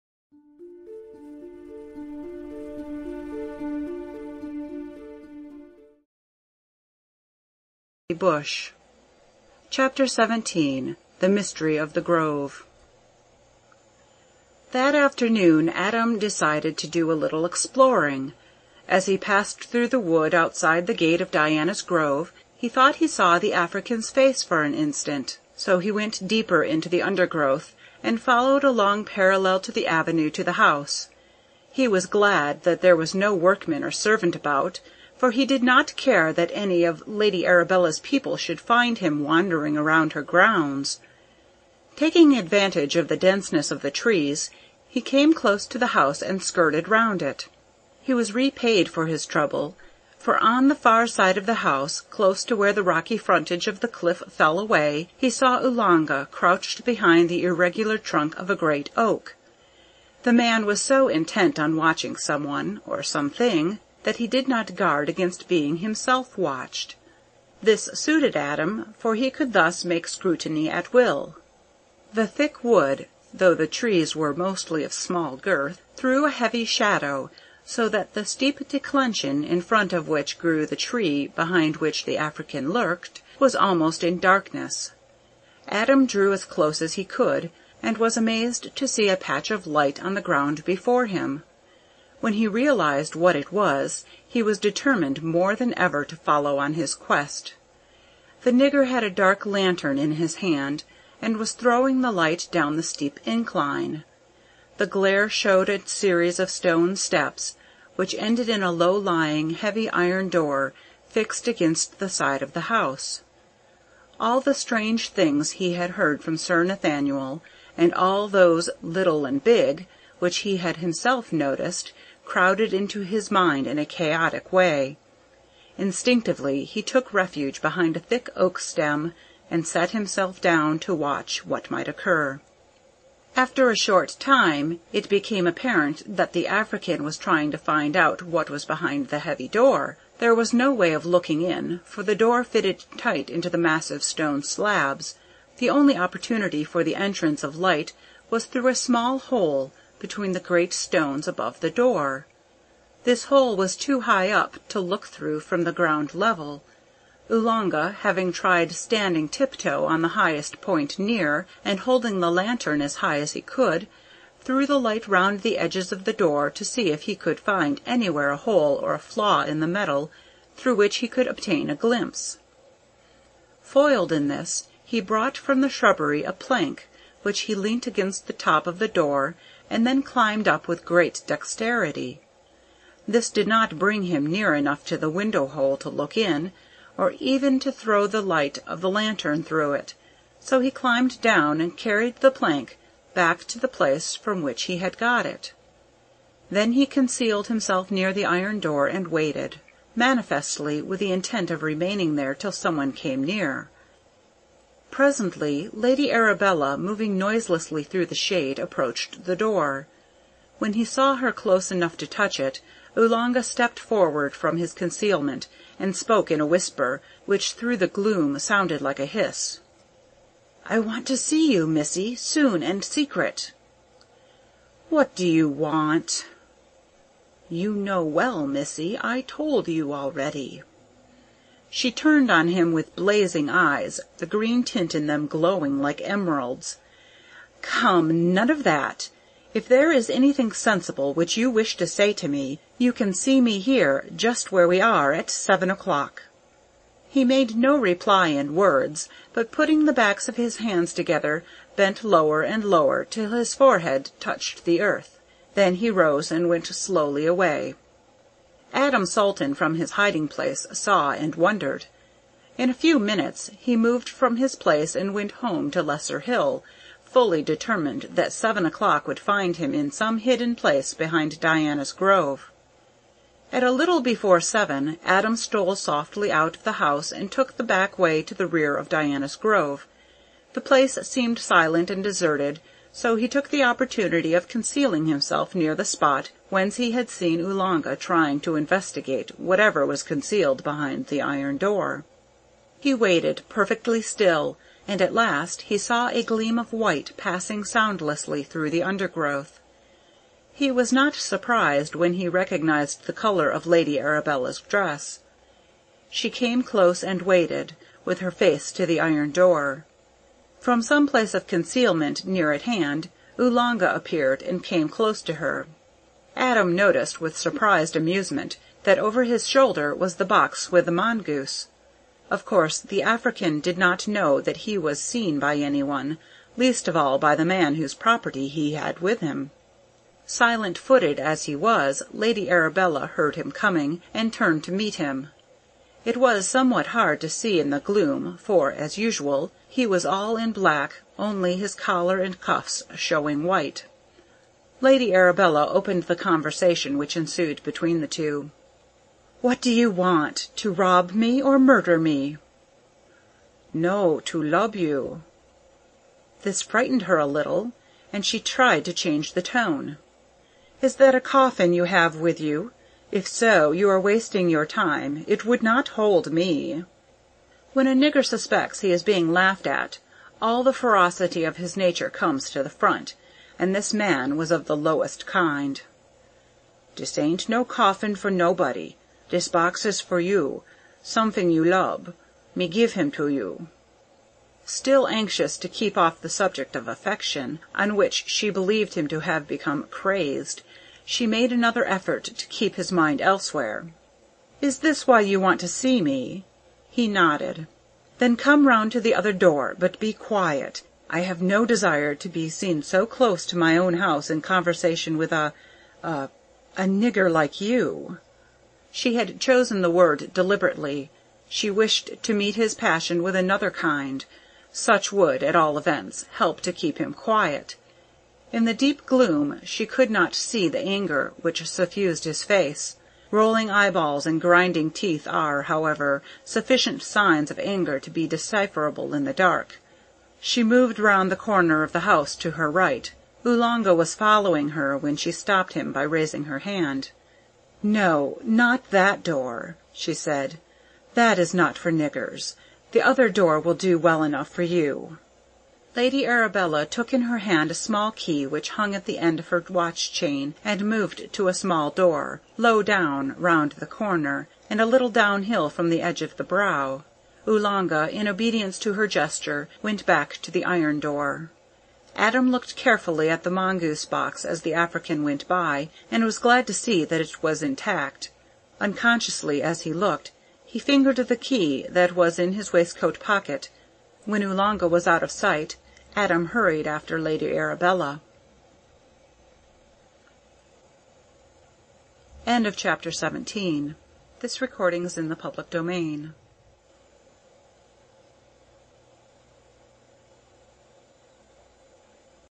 The Lair of the White Worm Audiobook Chapter 17
The Lair of the White Worm Audiobook Chapter 17 with subtitles in English by Bram Stoker. Free full length audiobook with text.